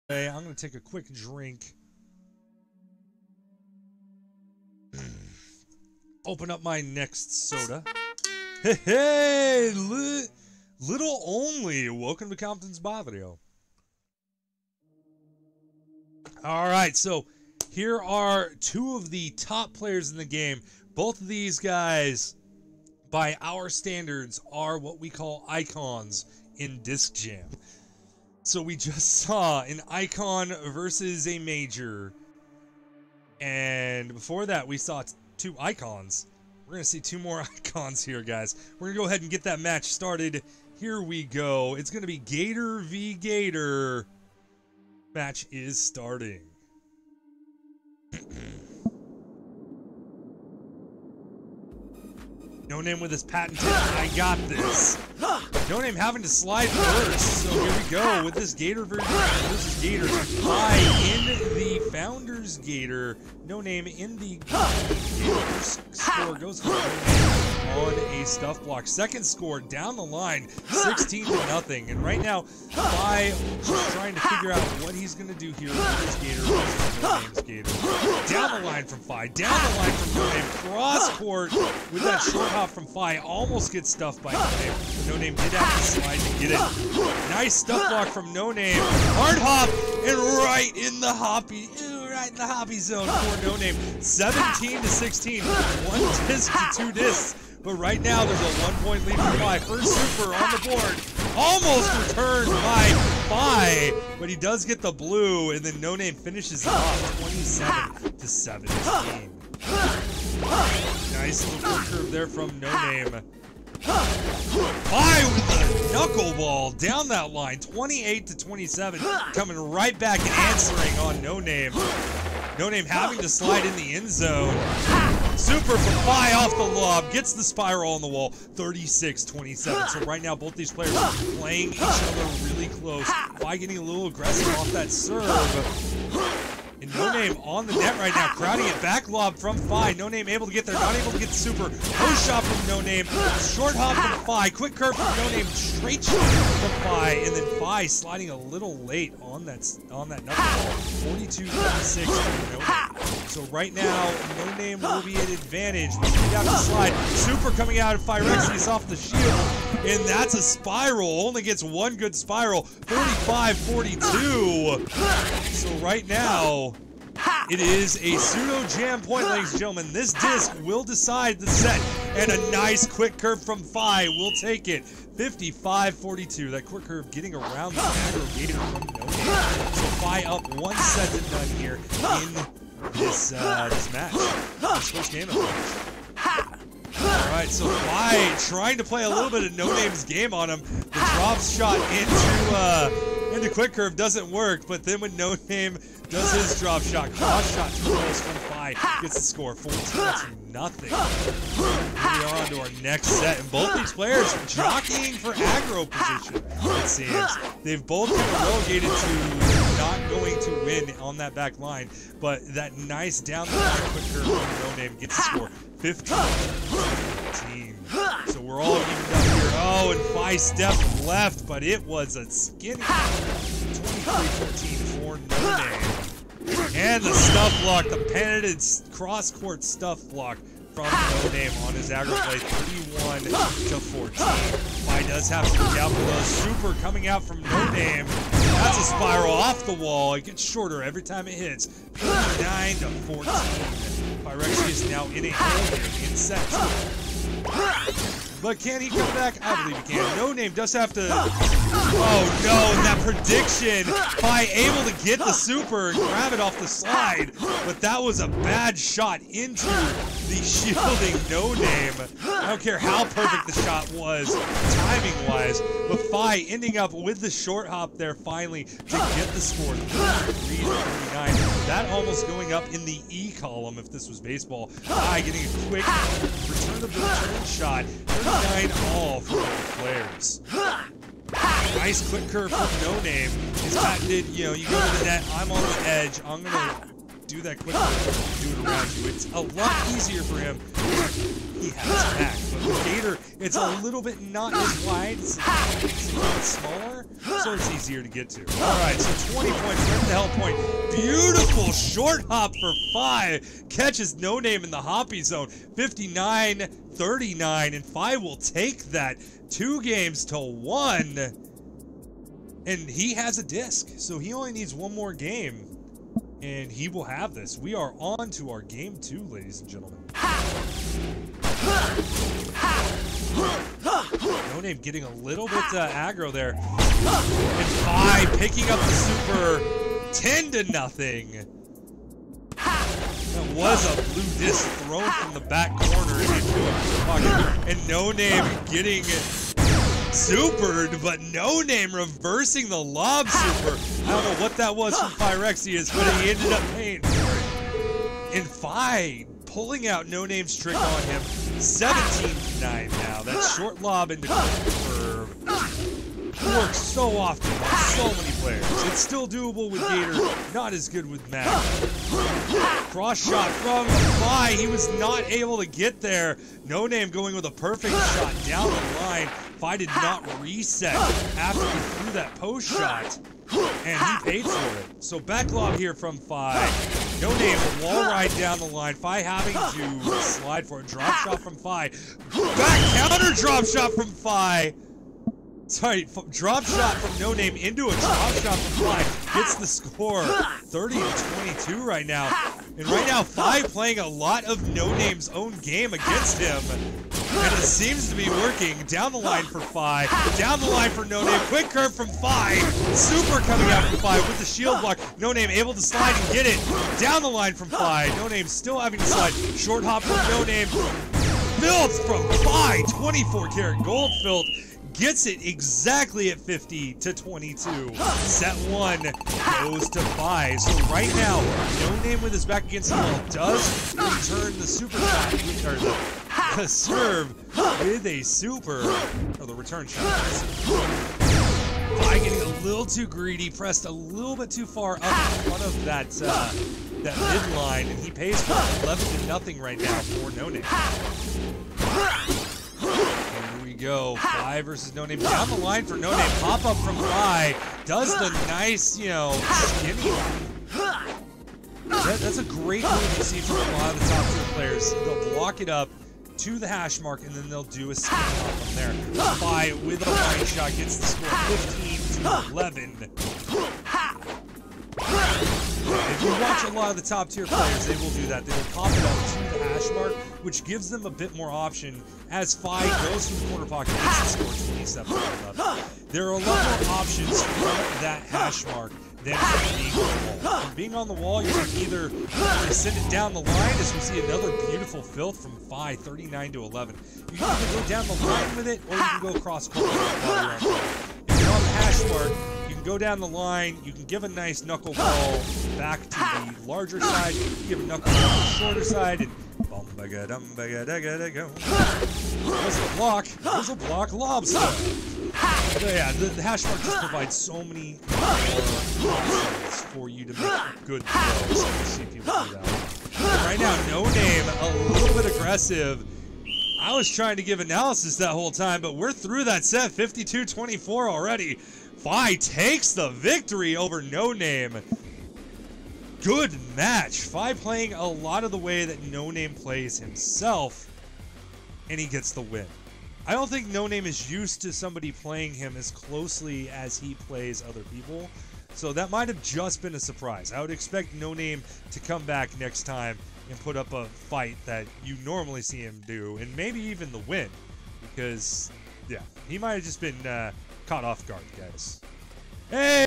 Throwdown Thursday: Phyrexius vs No_Name (Sept 13th 2018)
Our #2 clip of the night is a matchup between two more Icons in Phyrexius and No_Name.
Commentary: ComptonEMT